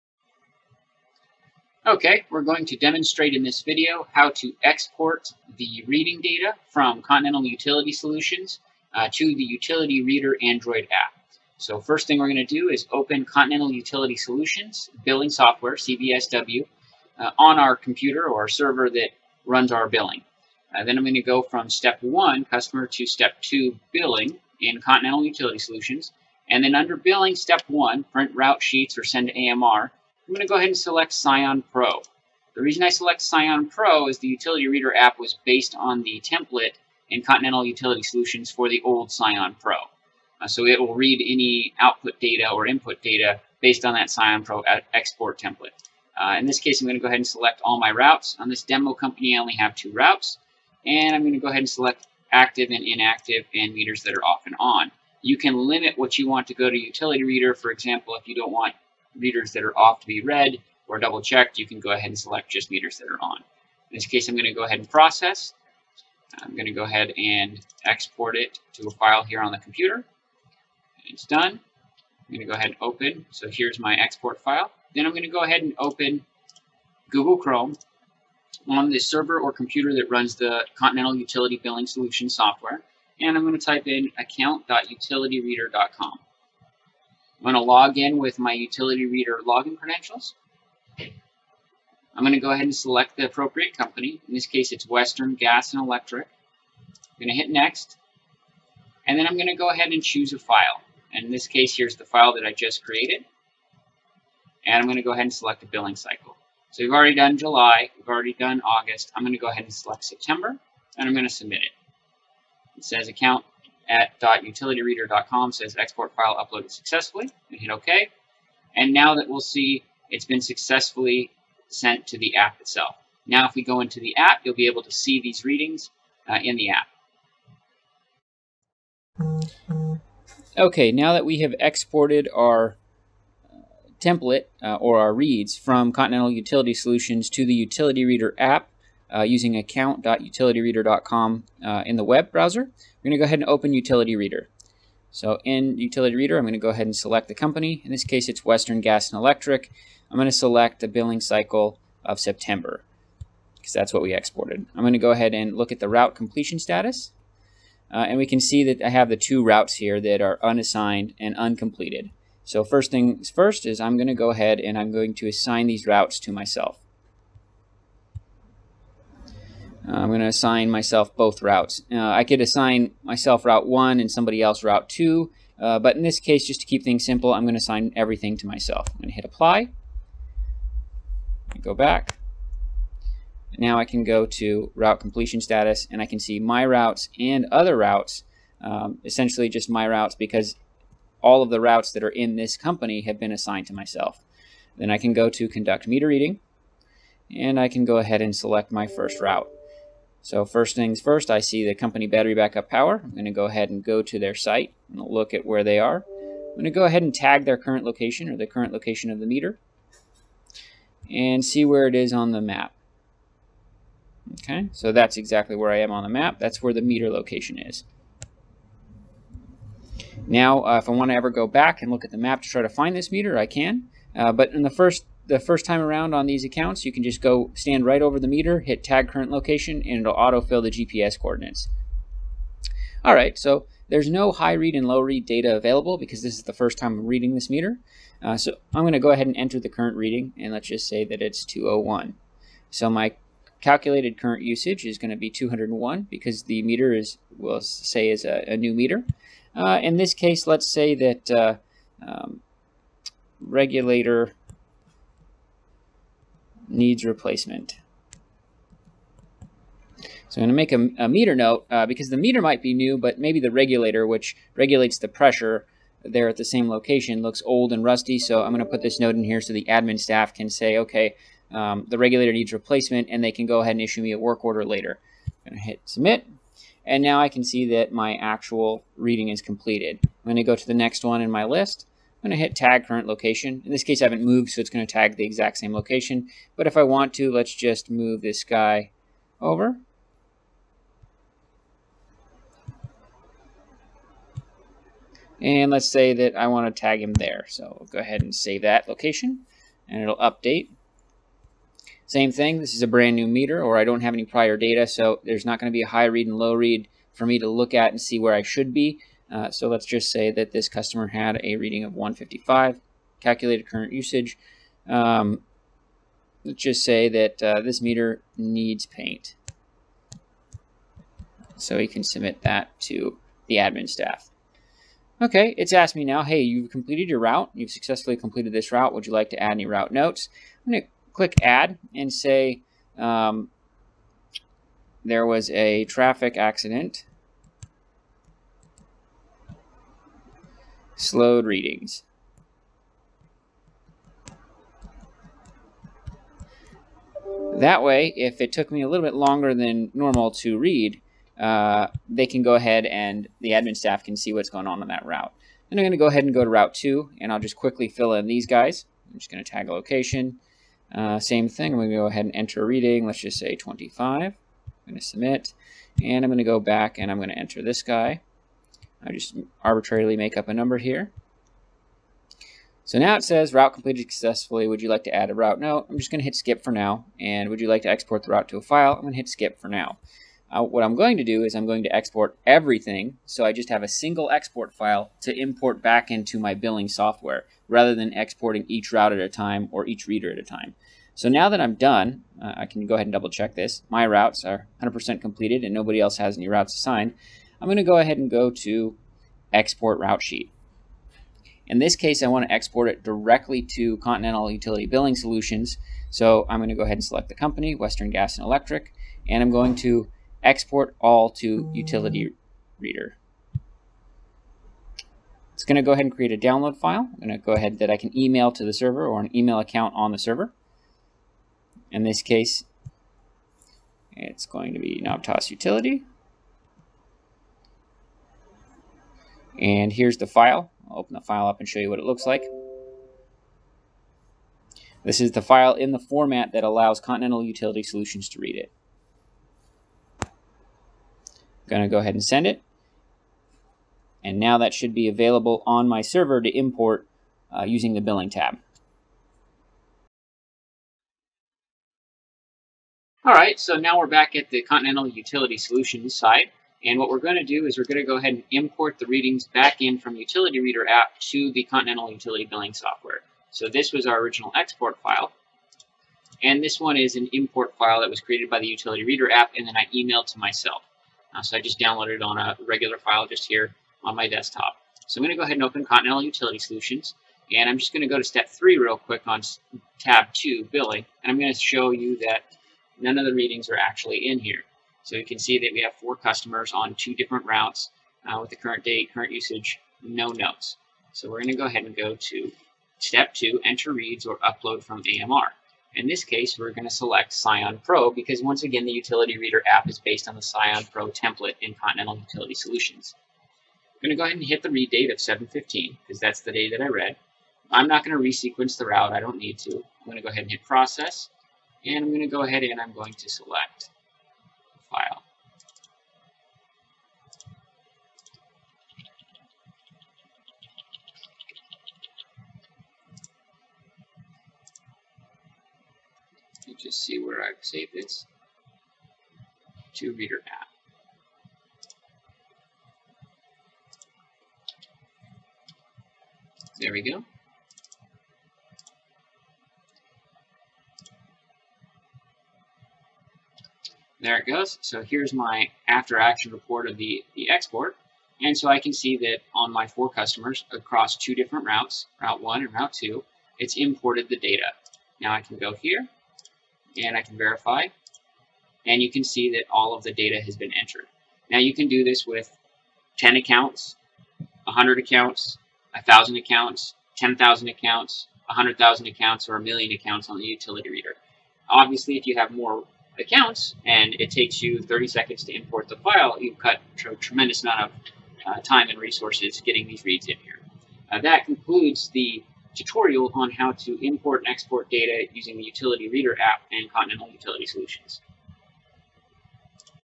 How To Use The Utility Reader App With CUSI Or CBSW
This video demonstrates how to use the Utility Reader Android app with CUSI (Continental Utility Solutions Inc). It covers exporting the meter data from CUSI's CBSW utility billing software, using the utility reader app to enter readings in the field, using the utility reader app to get GPS coordinates for every meter, and importing the data back into CUSI's CBSW utility billing software. The Utility Reader app has been proven to slash time, costs, and employee dissatisfaction associated with meter reading.